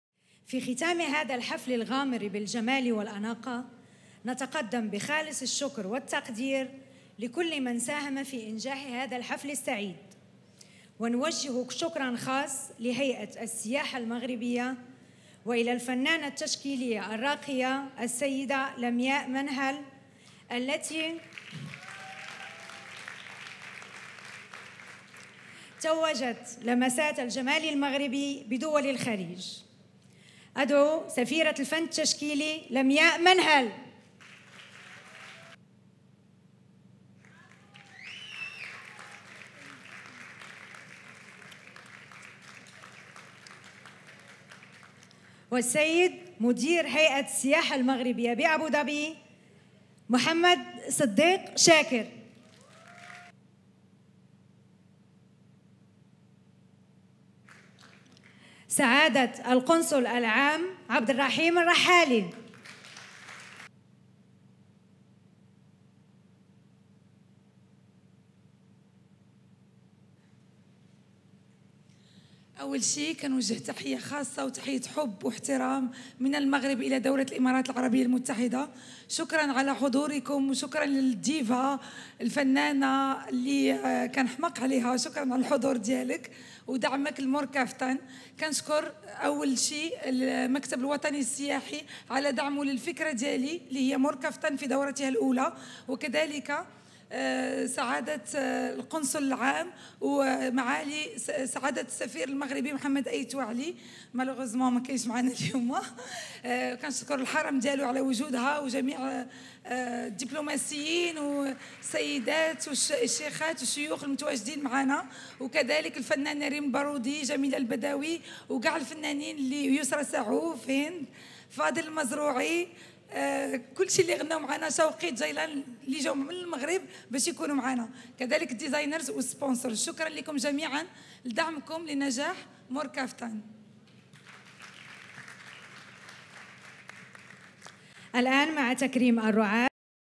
Speech LAMIAA Menhal at Morcaftan 1st ed at Four Season hotel Dubai Dec 2018
Izil
Taif al emarat
Ameera London
Marassim
Laylati wedding events
NIRVANA TRAVEL
Luxuxry explorer events